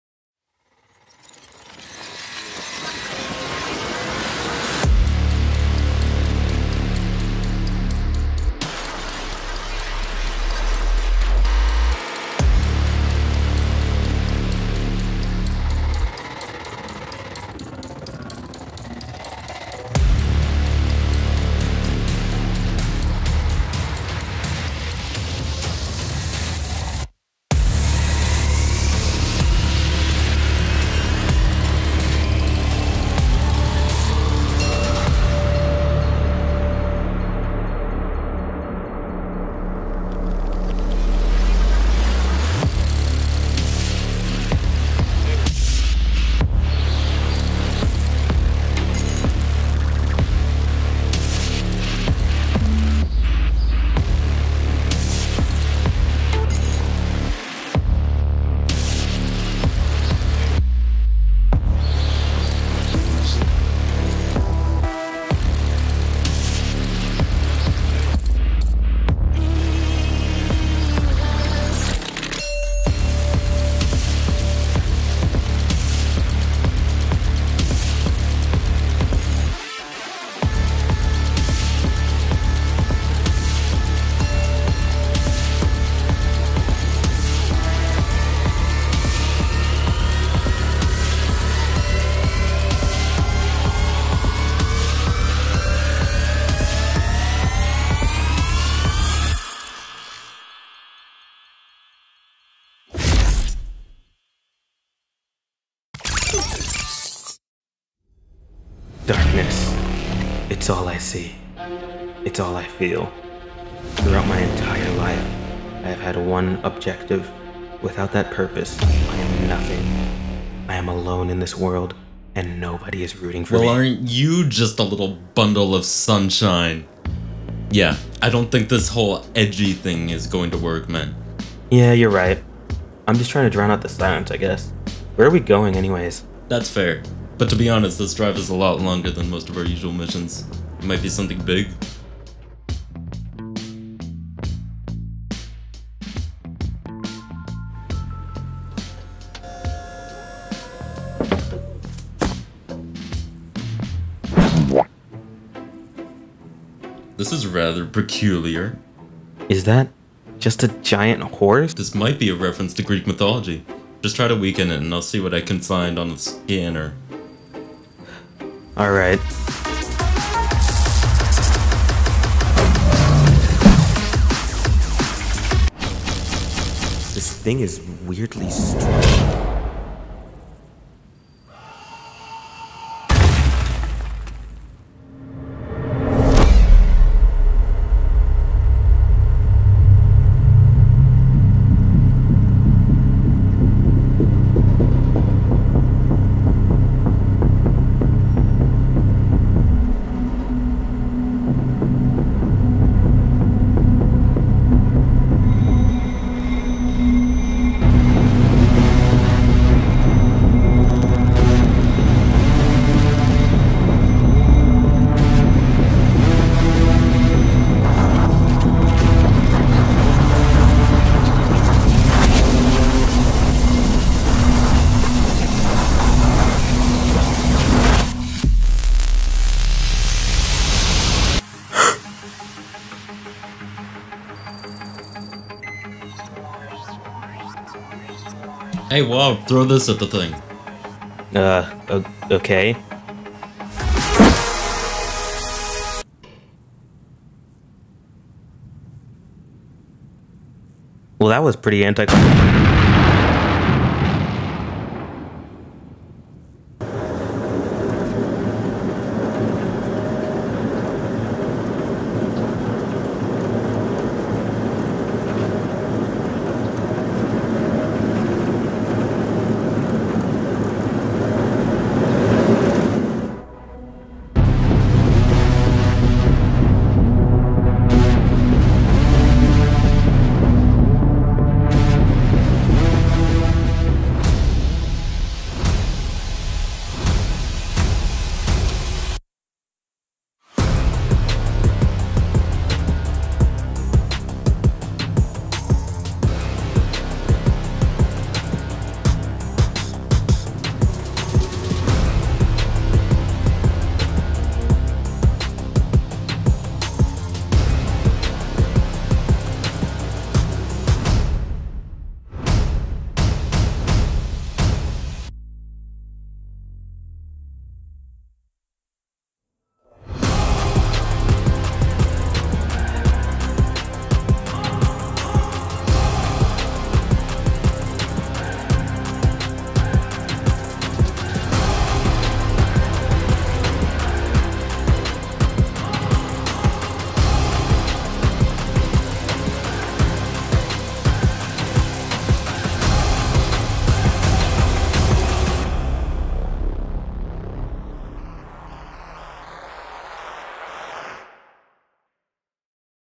Project Waup
3D Animation TV Pilot about a duo anti-hero science experiment that leads them to face off with a big horse.